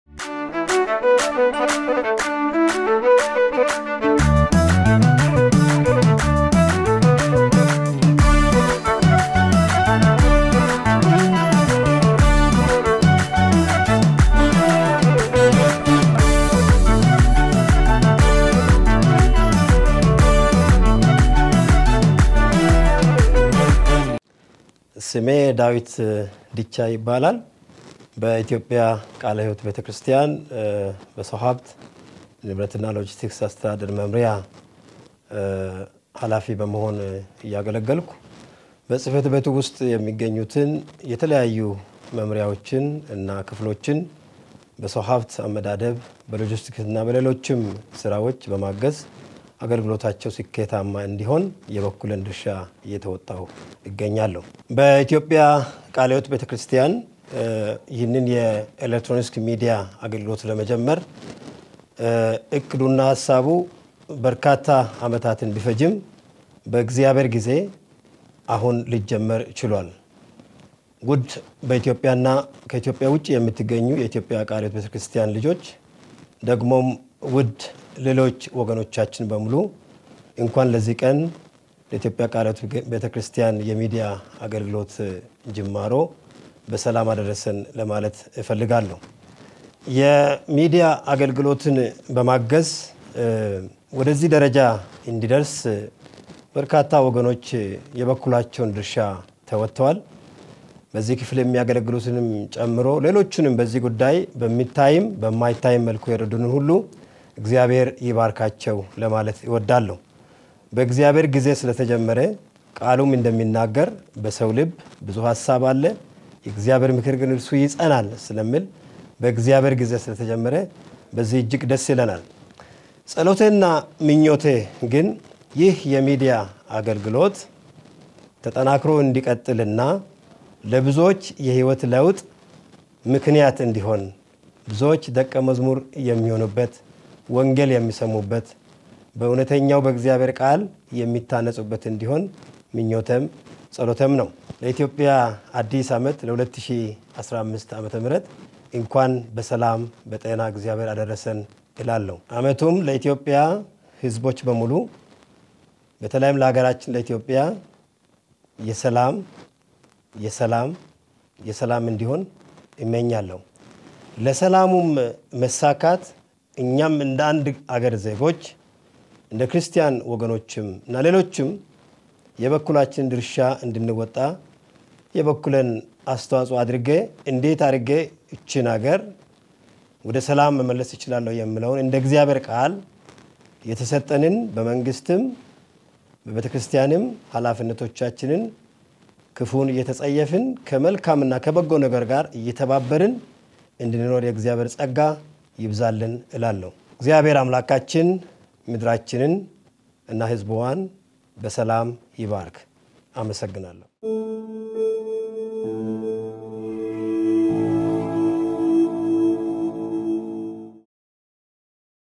Dawit Dicha
New Year message from the Ethiopian Kale Heywet Church leaders and staffs.